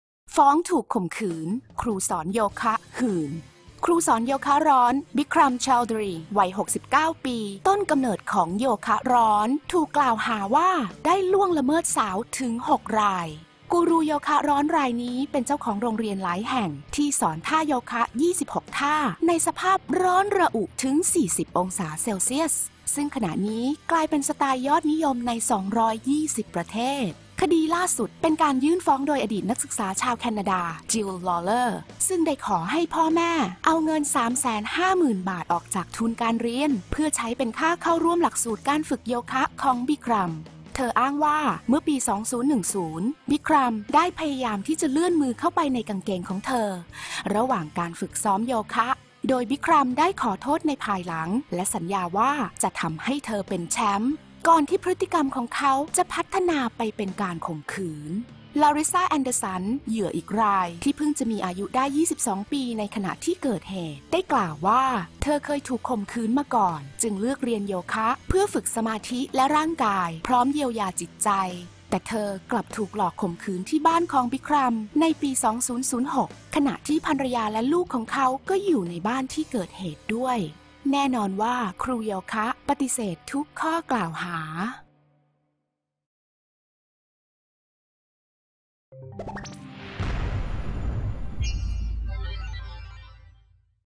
ฟ้องถูกข่มขืน ครูสอนโยคะหื่น
US -- ครูสอนโยคะร้อน Bikram Choudhury วัย 69 ปี ต้นกำเนิดของโยคะร้อน ถูกกล่าวหาว่าล่วงละเมิดทางเพศสาวหกราย

กูรูโยคะร้อนรายนี้เป็นเจ้าของโรงเรียนหลายแห่ง ที่สอนท่าโยคะ 26 ท่า ในสภาพร้อนระอุถึง 40 องศาเซลเซียส ซึ่งเป็นสไตล์ยอดนิยมใน 220 ประเทศ

คดีล่าสุดเป็นการยื่นฟ้องโดยอดีตนักศึกษาชาวแคนาดา จิลล์ ลอว์เลอร์ ซึ่งได้ขอให้พ่อแม่เอาเงิน 350,000 บาทออกจากทุนเรียน เพื่อเป็นค่าเข้าร่วมหลักสูตรการฝึกโยคะของบิครัม

เธออ้างว่าเมื่อปี 2010 บิครัมได้พยายามเลื่อนมือข้าไปในกางเกงของเธอระหว่างการฝึกซ้อม โดยบิครัมได้ขอโทษในภายหลัง และสัญญาว่าจะ "ทำให้เธอเป็นแชมป์" ก่อนที่พฤติกรรมของเขาจะพัฒนาไปเป็นการข่มขืน

ลาริสซา แอนเดอร์สัน เหยื่ออีกราย ที่เพิ่งจะอายุได้ 22 ปีขณะเกิดเหตุ กล่าวว่าเธอเคยถูกข่มขืนมาก่อน จึงเลือกเรียนโยคะเพื่อฝึกสมาธิและร่างกาย พร้อมเยียวยาจิตใจ  แต่เธอกลับถูกหลอกข่มขืนที่บ้านของเขาในปี 2006 ขณะที่ภรรยาและลูกของเขาก็อยู่ในบ้านที่เกิดเหตุด้วย

แน่นอนว่าครูโยคะปฏิเสธทุกข้อกล่าวหา